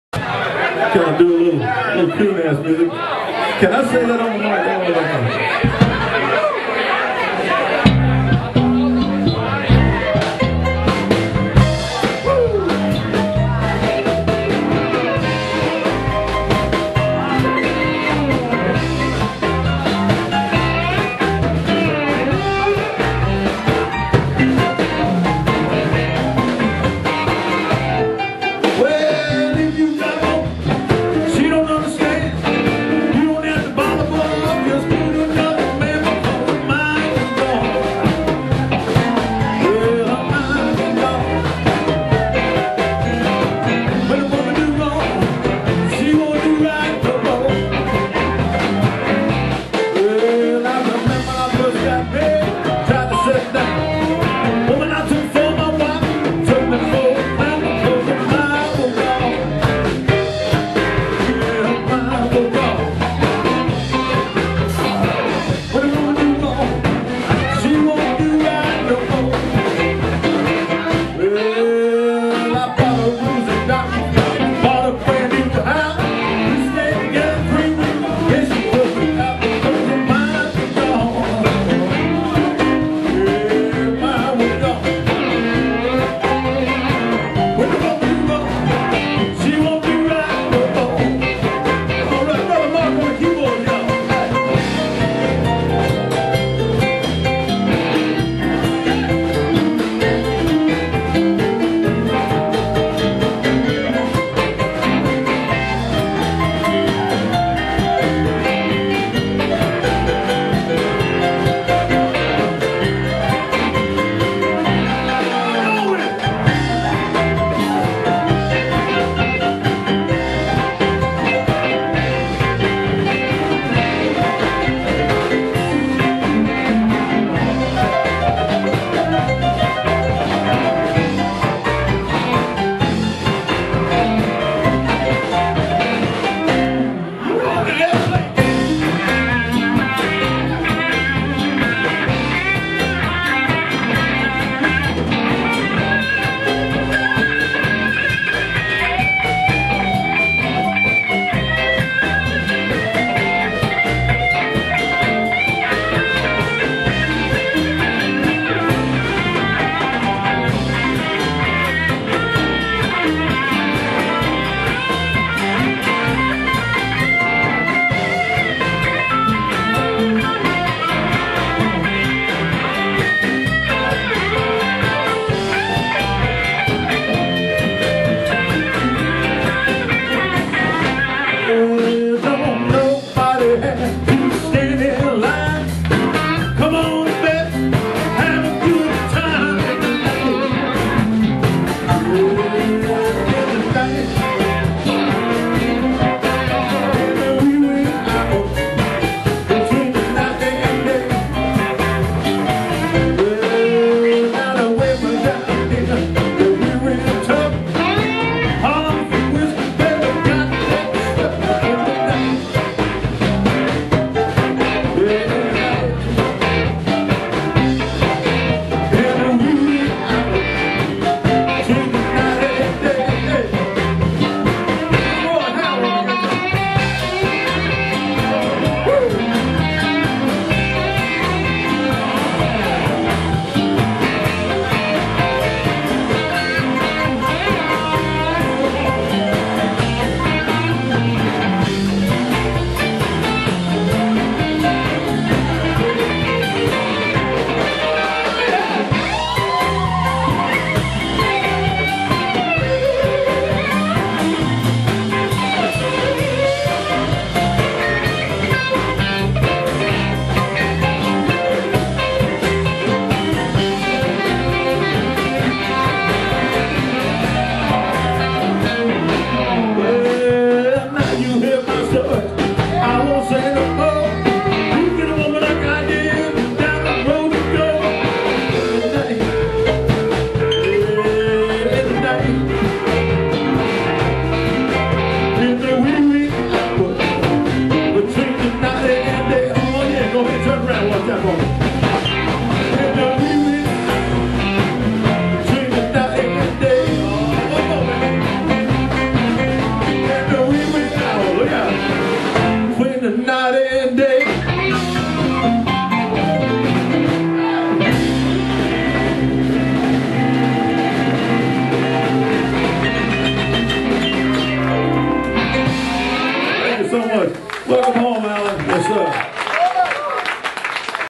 ROY HOWELL..East End Grill...Memphis jam w/ 'Groove' Parker...June 16, 2011
Jamming were: Mark Ross (Keys), 'Groove' Parker (Bass and Vocals), Alan Copeland (Drums), Evan Leake (Guitar), Roy Howell (Slide guitar).